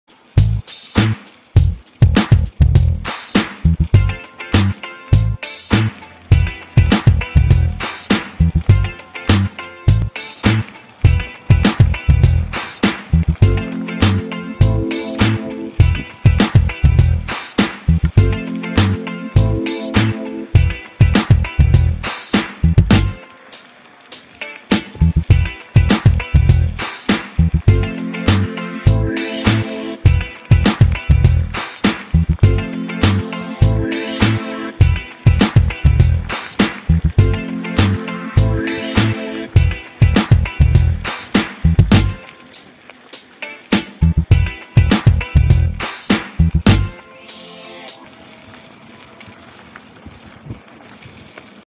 Cool ride
Don and i went for an early morning ride.